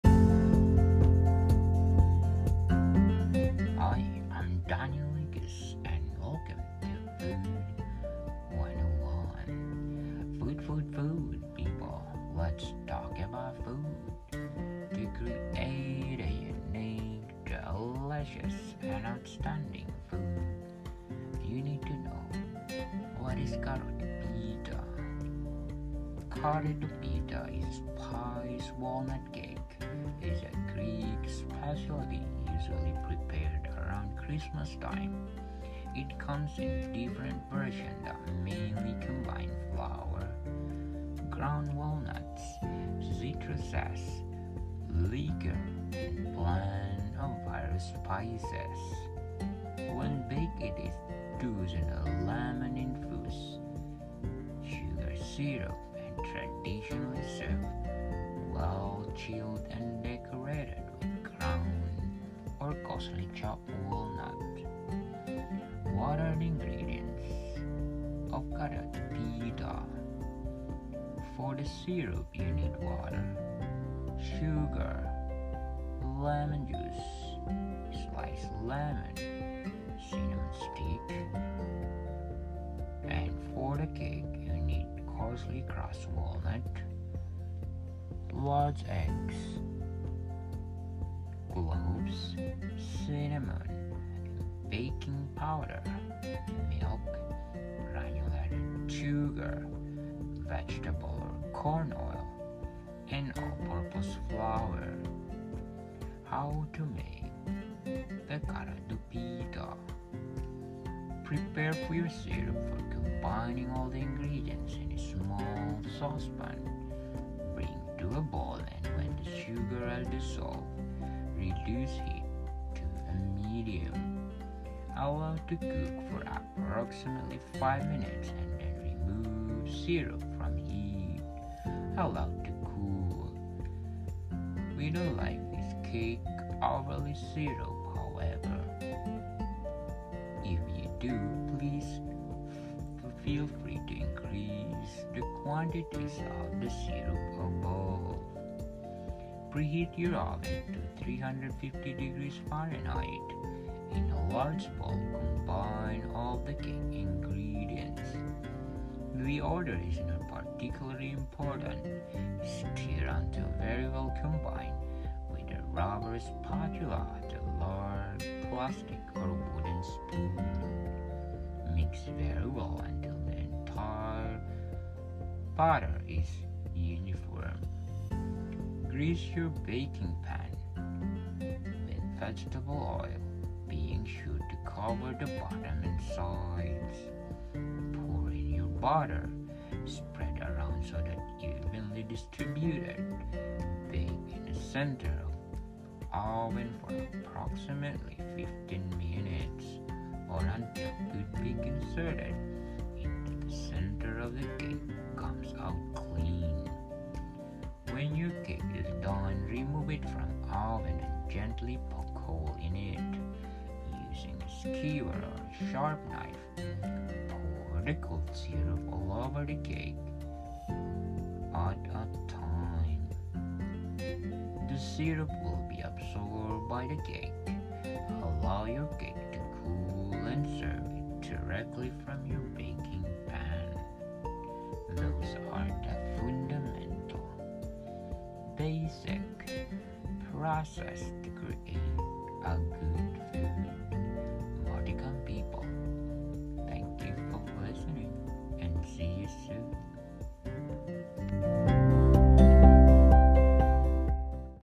Karydopita
What is Karydopita and key ingredients in Greek Cuisine